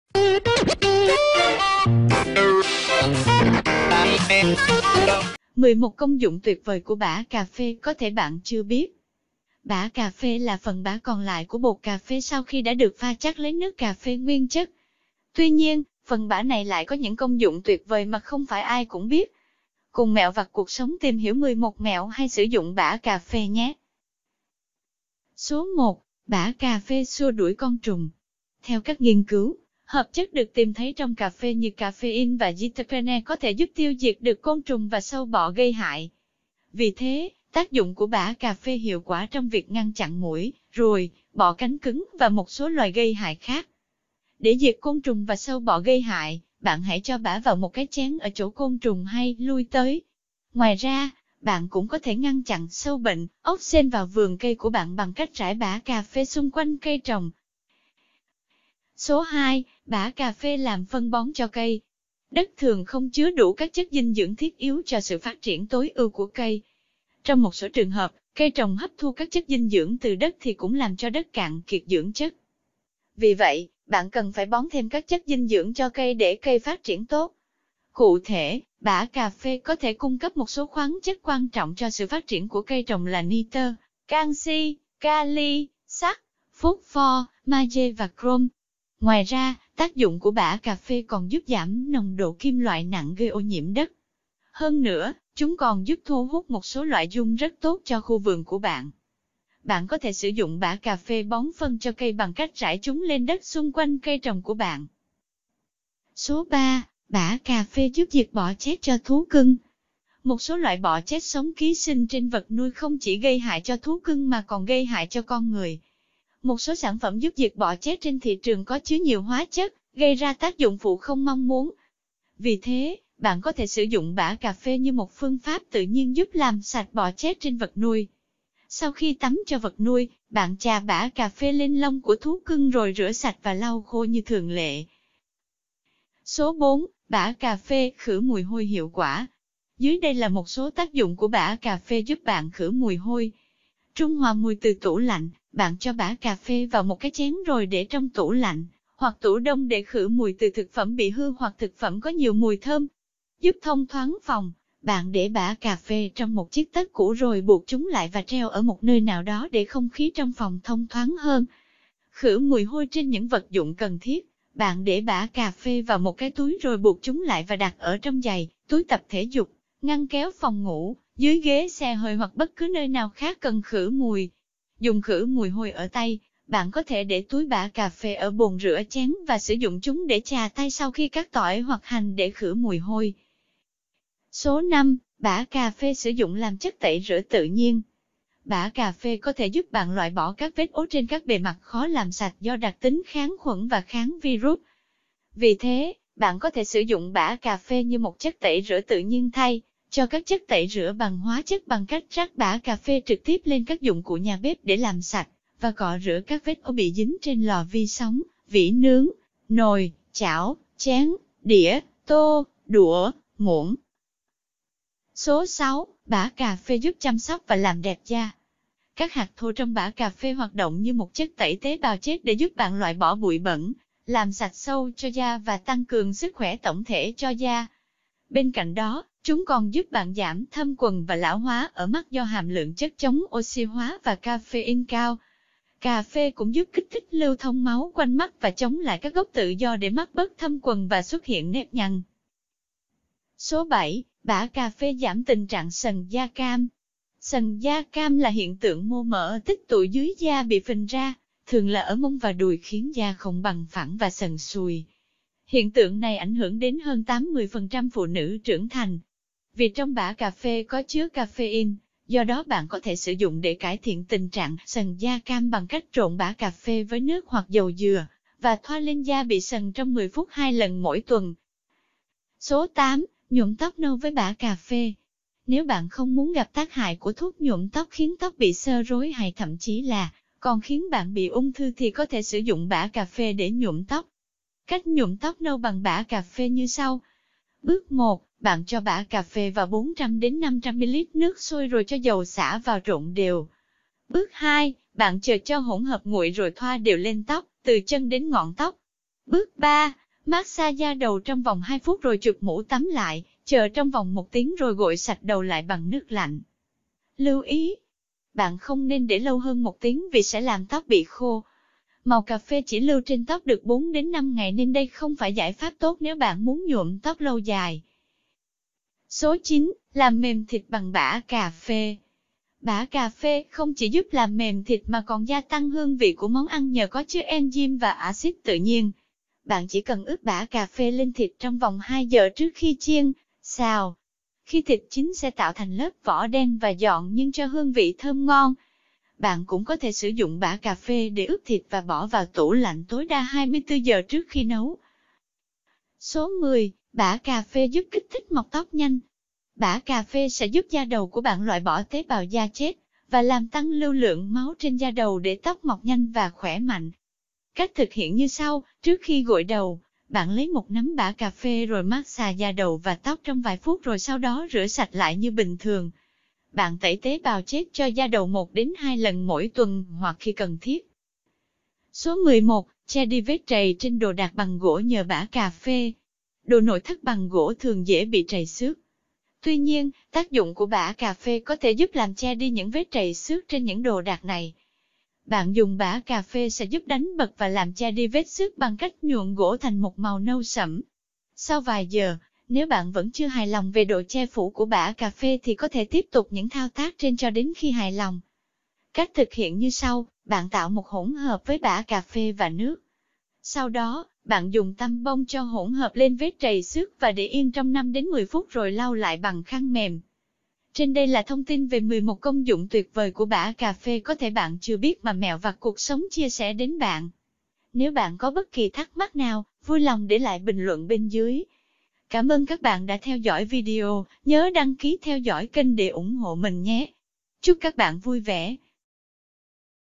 Công dụng bã cà phê tuyệt vời có thể bạn chưa biết - Mẹo Vặt Cuộc Sống
Bã cà phê là phần bã còn lại của bột cà phê sau khi đã được pha chắt lấy nước cà phê nguyên chất. Tuy nhiên, phần bã này lại có những công dụng tuyệt vời mà không phải ai cũng biết. Cùng mẹo vặt cuộc sống tìm hiểu 11 mẹo hay sử dụng bã cà phê nhé

Hastags